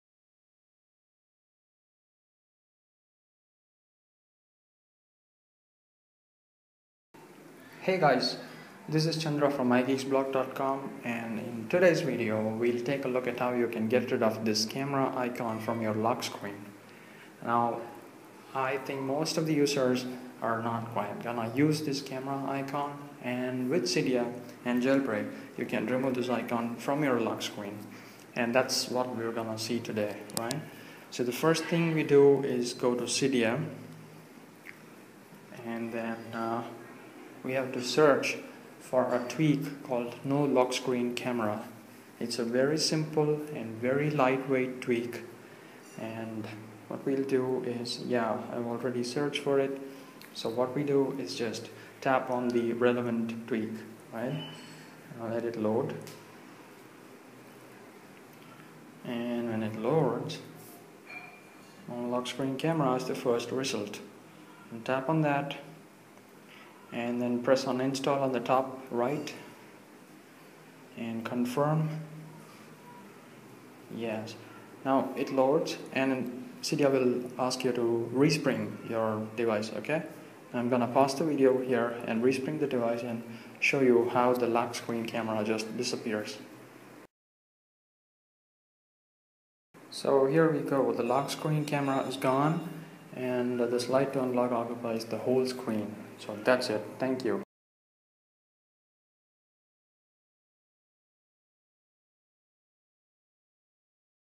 How to Remove Camera Icon from iPhone Lockscreen
This quick video tutorial walks you through how to remove camera option from the lockscreen of your jailbrokn iPhone or iPod Touch. You just need to install "No Lockscreen Camera" cydia tweak to remove camera icon from your iPhone's lockscreen.